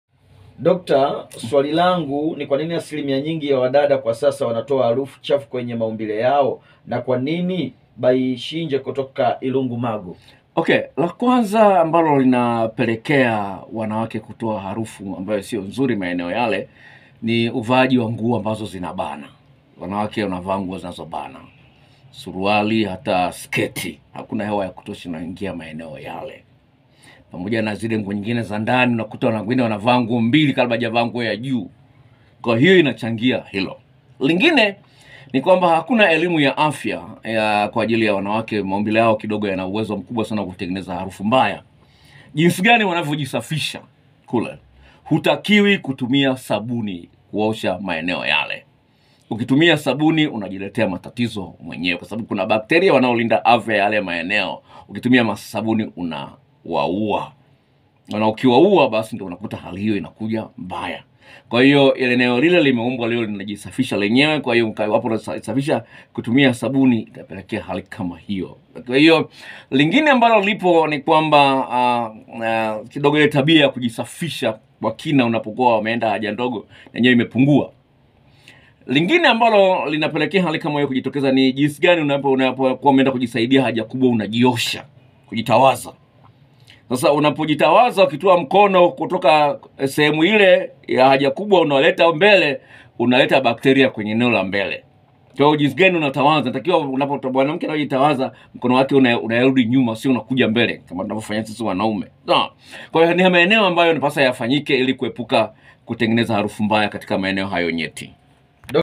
Mwanamke mwenye harufu mbaya ukkeni aepuke mambo haya 4
HUWEZI KUAMBIWA UNANUKA LAKINI ITAKUGHARIMU